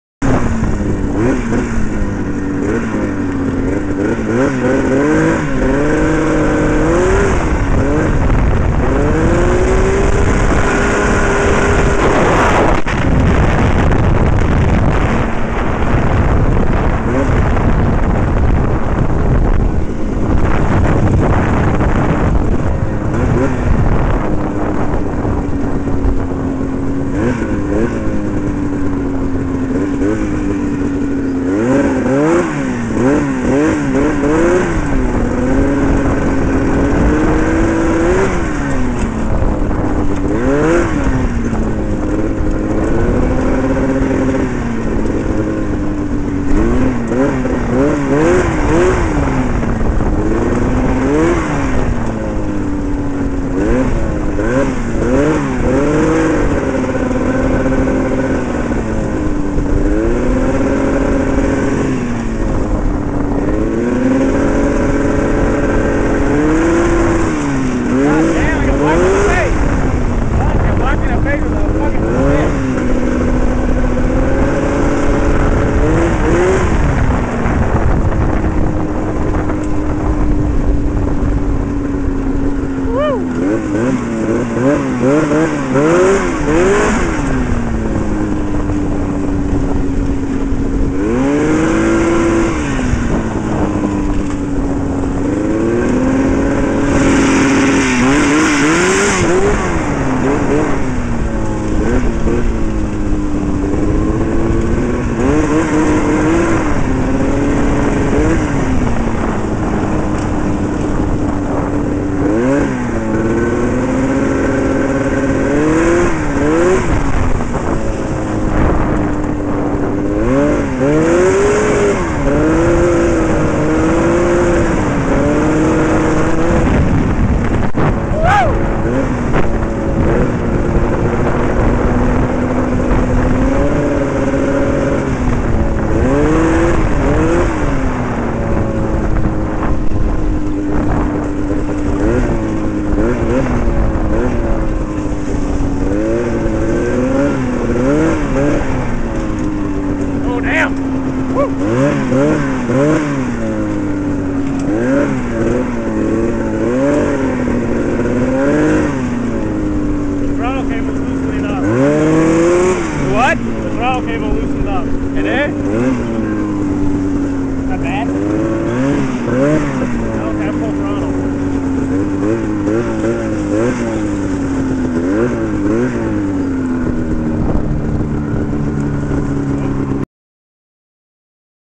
Two Stroke go kart onboard
my summer go kart project. The engine is from a 79 arctic cat el tigre 5000.